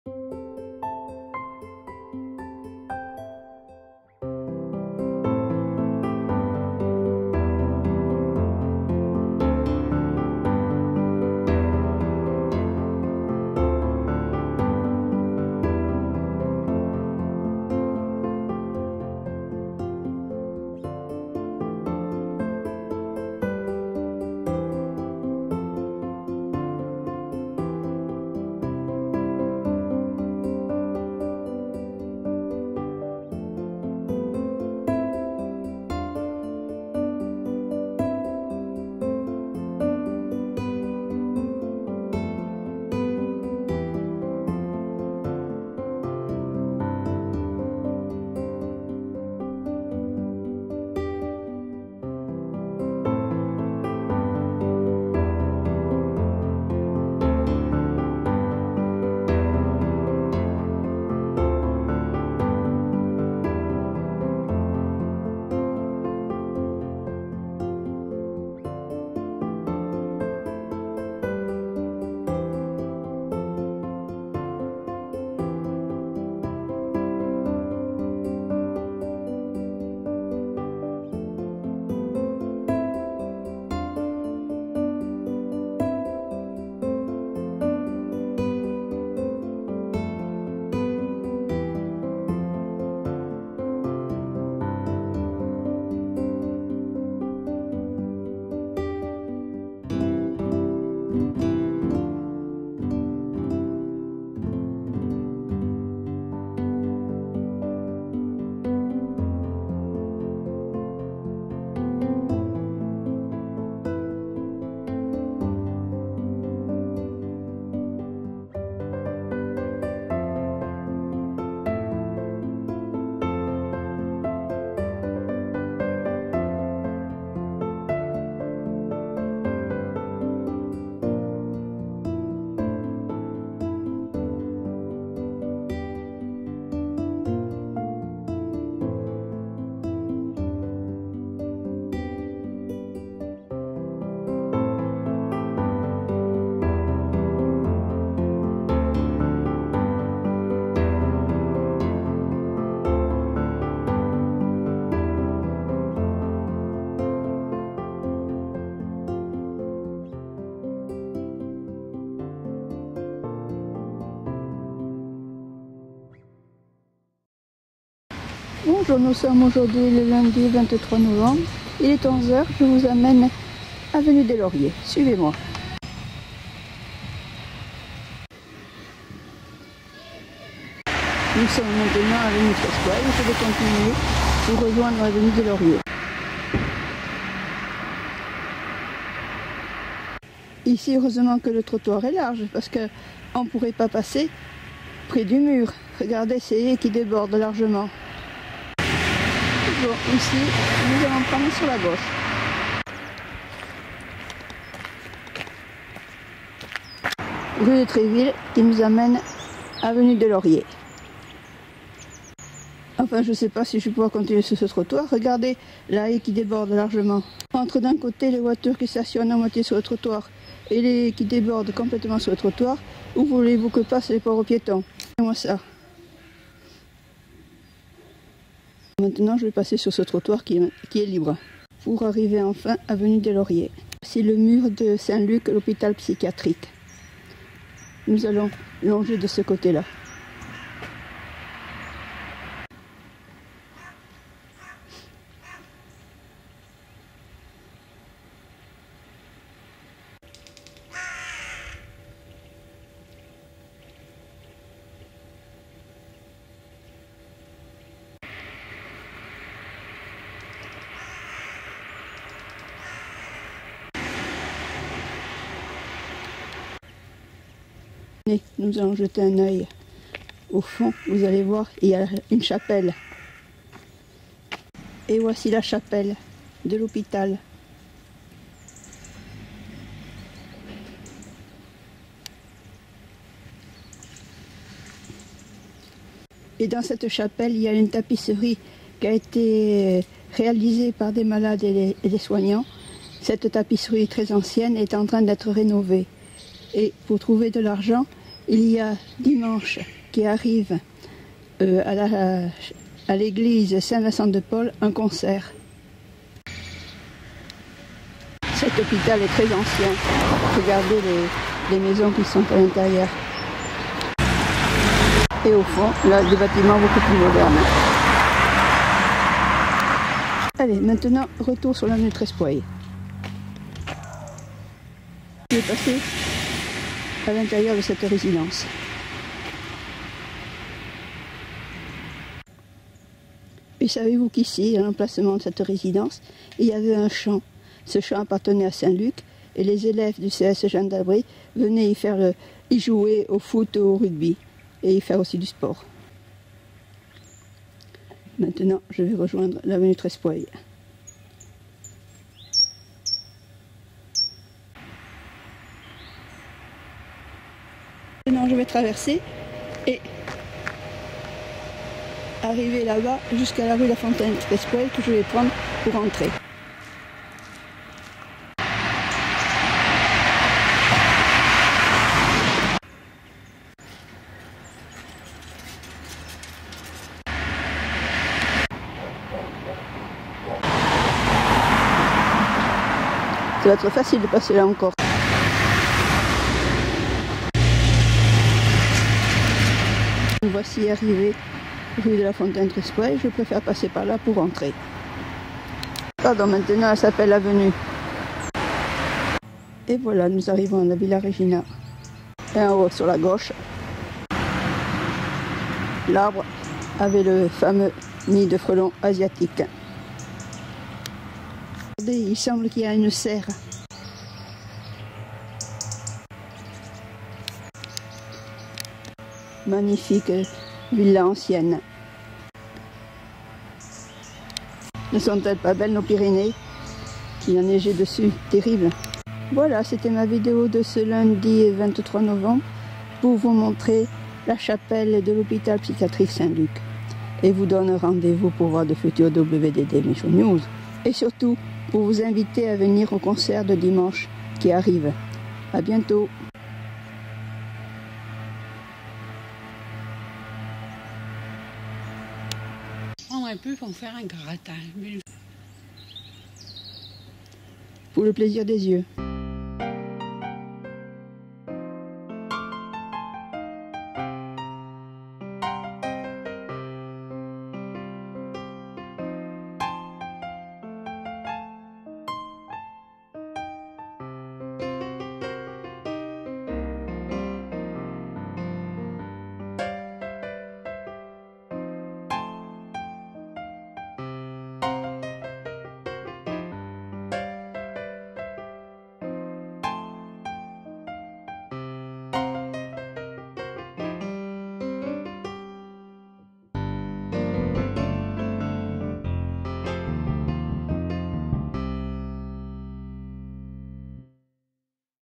LES INVITATIONS DE MICHOU W D D 23 NOVEMBRE 2015 PAU VENEZ ASSISTER AU CONCERT DU 29 NOVEMBRE
Bonjour,
viens vous inviter à assister ce dimanche 29 novembre à un concert donné en l'Eglise SAINT VINCENT DE PAUL (de Pau) à 16 h. Ce concert est donné pour aider " LES AMIS DE LA CHAPELLE SAINT LUC " à financer la rénovation de la tapisserie et de la chapelle de SAINT LUC hôpital psychiatrique de Pau.
A dimanche !
Le site des amis de la chapelle Saint LUC
Pour aider à la rénovation
autre site : aumoneriehopitauxpau.fr/